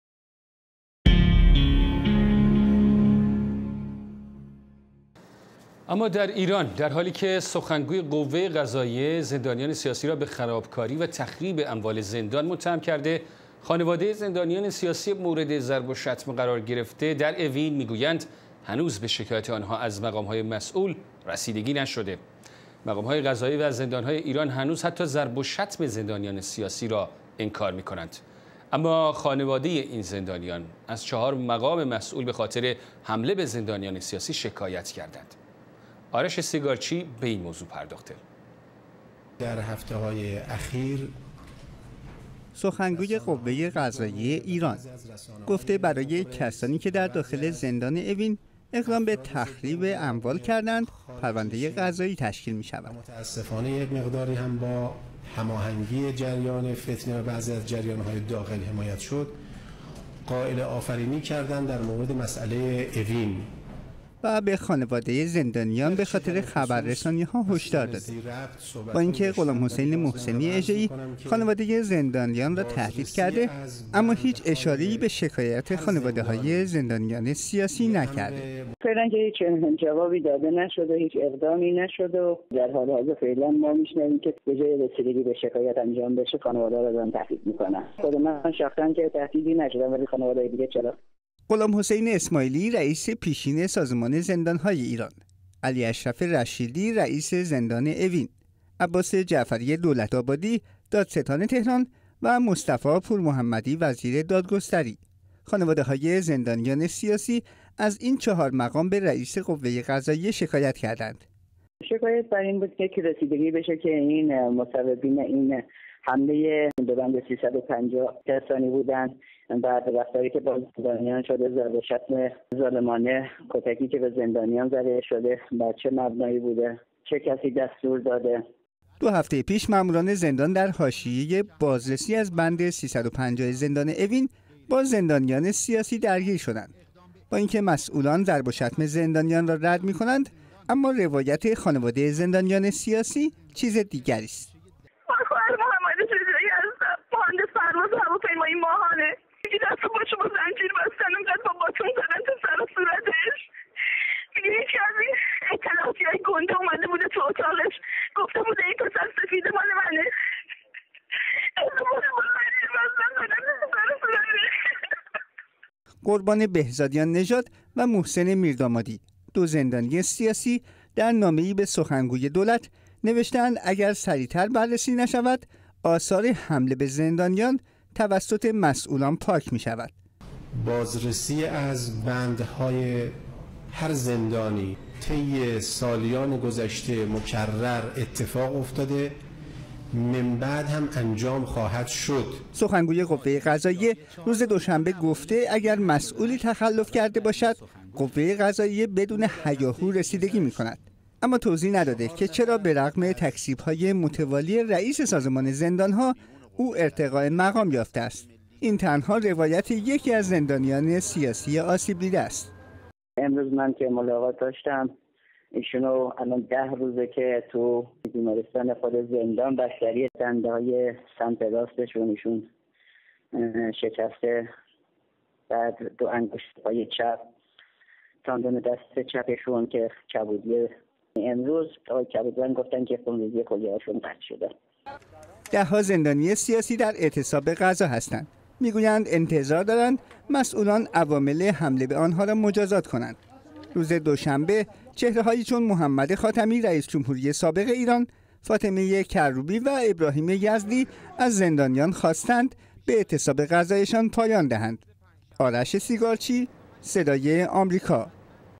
محسنی اژه ای تهدید کرد؛ خانواده های زندانیان اوین شکایت کردند
در حالیکه سخنگوی قوه قضاییه ایران، زندانیان سیاسی را به خرابکاری و تخریب اموال زندان متهم می کند، خانواده زندانیان سیاسی مورد ضرب و شتم قرارگرفته در زندان اوین، می گویند هنوز به شکایت آنها از مقام های مسوول رسیدگی نشده است.
مقام های قضایی و زندان های ایران هنوز ضرب  و شتم زندانیان سیاسی را رد می کنند اما خانواده این زندانیان از چهار مقام مسوول بخاطر حمله به زندانیان سیاسی شکایت کرده اند.
آرش سیگارچی گزارش می دهد: